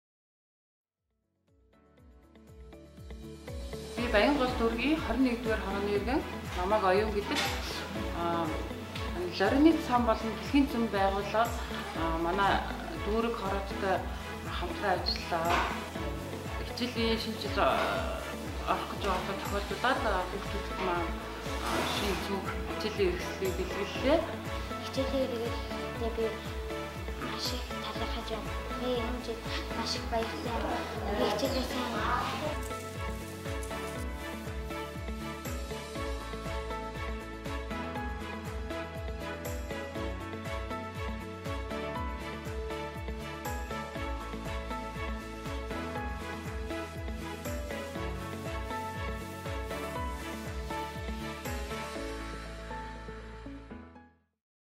In partnership with Lorinet Foundation, we have delivered school supplies to 1000 children.
Thursday Back to school video –
In partnership with Lorinet Foundation, we have delivered school supplies to 1000 children back in September. The children started their school with excitement after months of quarantine, but today we shouldn’t forget our children who are not being able to access online education.
Thanks to Lorinet Foundation.
for their contribution to support